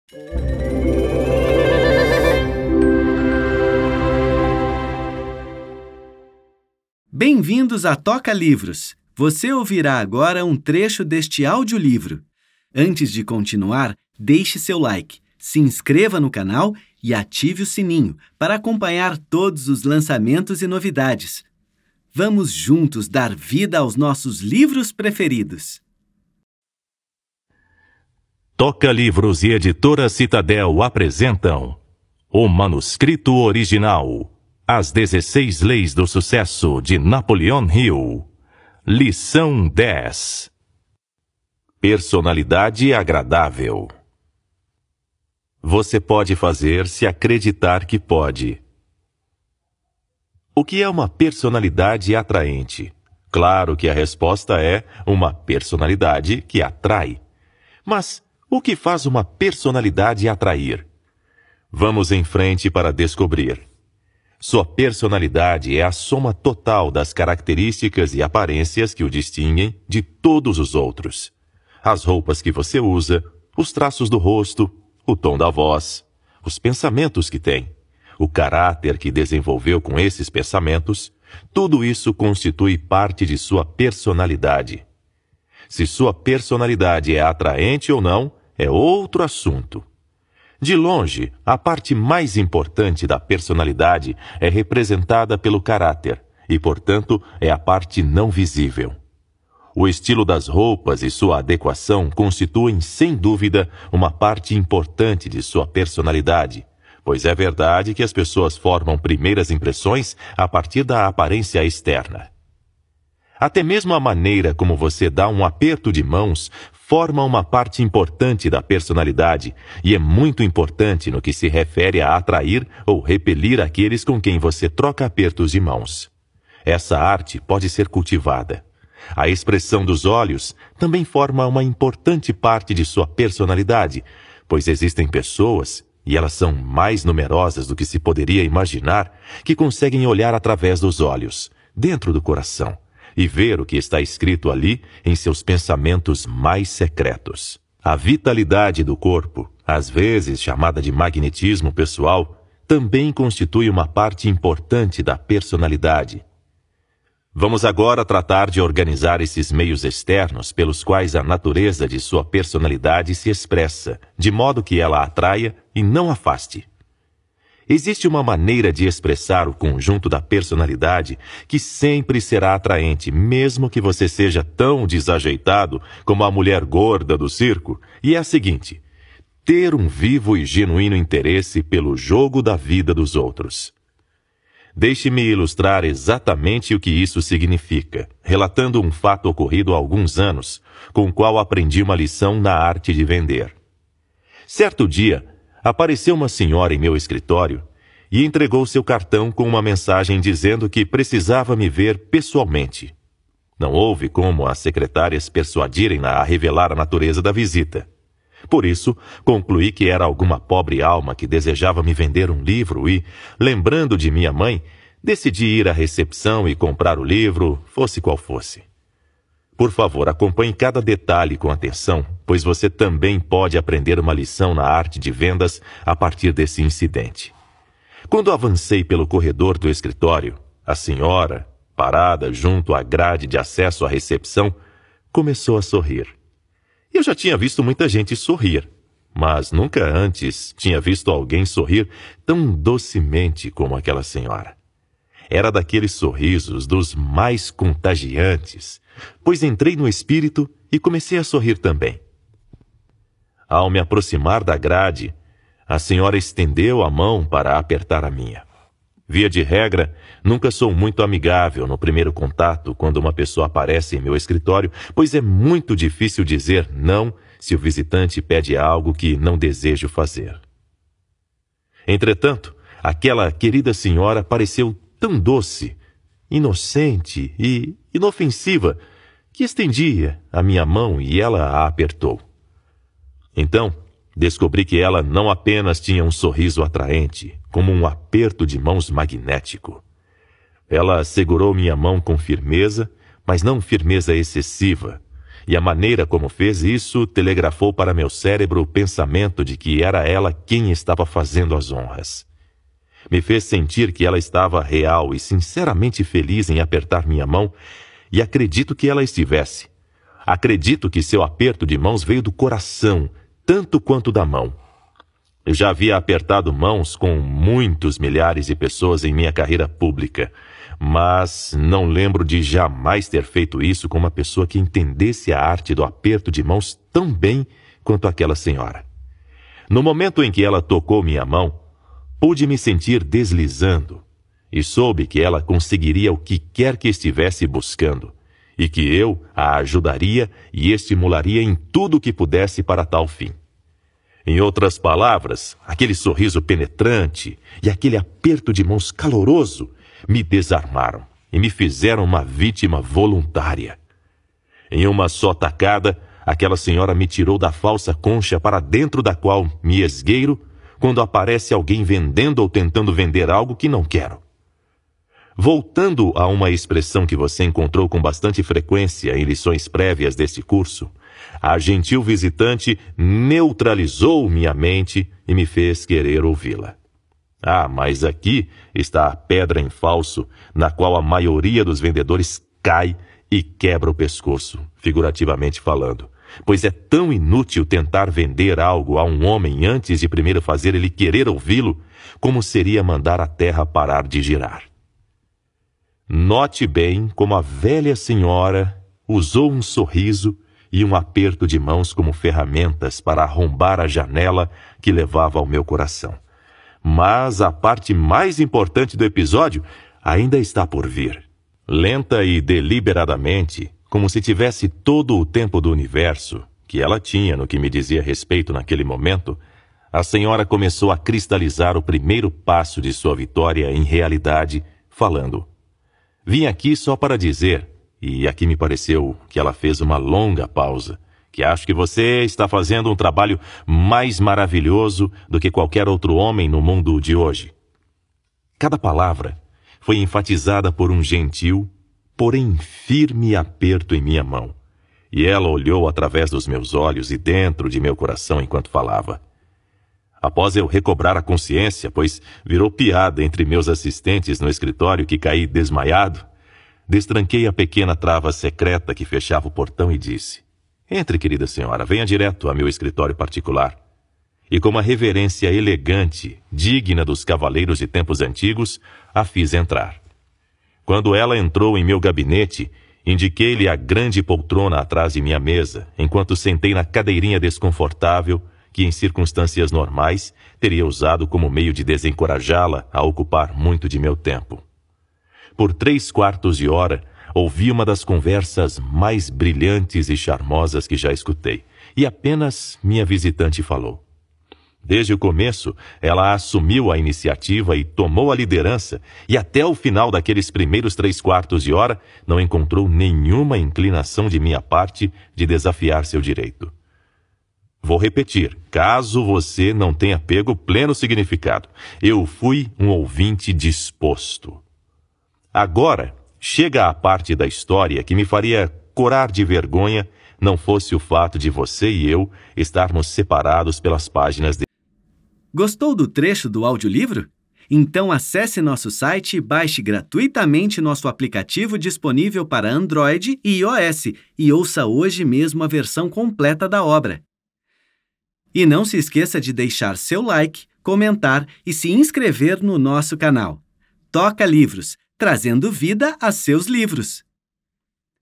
AUDIOLIVRO - PERSONALIDADE AGRADÁVEL - LIÇÃO 10: O MANUSCRITO ORIGINAL
Personalidade Agradável - Lição 10: O Manuscrito Original

Nessa lição vamos agora tratar de organizar esses meios externos pelos quais a naturezade sua personalidade se expressa, de modo que ela atraia e não afaste. Acompanhe cada detalhe com atenção, pois você também pode aprender esta nova lição.Esse plano pode ser colocado em operação por qualquer pessoa com pouca preparação.

O Manuscrito Original — As leis do triunfo e do sucesso de Napoleon Hill é a bíblia para a realização profissional em qualquer campo de atividade, reunindo o resultado de mais de vinte anos de pesquisas do autor.
Napoleon Hill entrevistou centenas dos homens mais ricos e bem-sucedidos de seu tempo, manteve contato próximo com vários deles, treinou exércitos de vendedores e também entrevistou mais de 15 mil pessoas para orientá-las na carreira profissional. Esse vasto conhecimento da natureza humana e as experiências pessoais de Hill como empreendedor fizeram dele um especialista em sucesso (e nas causas do fracasso também).
O manuscrito original ensina o que fazer para ser bem-sucedido na vida. Sucesso é mais do que acumular dinheiro e exige mais do que uma mera vontade de chegar lá. Napoleon Hill explica didaticamente como pensar e agir de modo positivo e eficiente e como conseguir a ajuda dos outros para a realização de objetivos. Cada capítulo do Manuscrito original aborda um tema específico, seguindo uma ordem evolutiva cuidadosa. Napoleon Hill (1883–1970), autor dos best-sellers Pense e enriqueça ( Think and Grow Rich ), Mais esperto que o Diabo ( Outwitting the Devil ) e Atitude mental positiva ( Success through a Positive Mental Attitude ), foi pioneiro na literatura sobre realização pessoal e é considerado um dos maiores e melhores escritores sobre sucesso de todos os tempos. Suas obras são lidas e apreciadas até hoje como manuais efetivos para quem busca sucesso na carreira profissional e nas relações sociais em geral. O manuscrito original (no original, The Law of Success in Sixteen Lessons ) é sua obra de maior fôlego e um clássico do gênero.
Ficha técnica

Autor: Napoleon Hill
Narrador: Daniel Vidal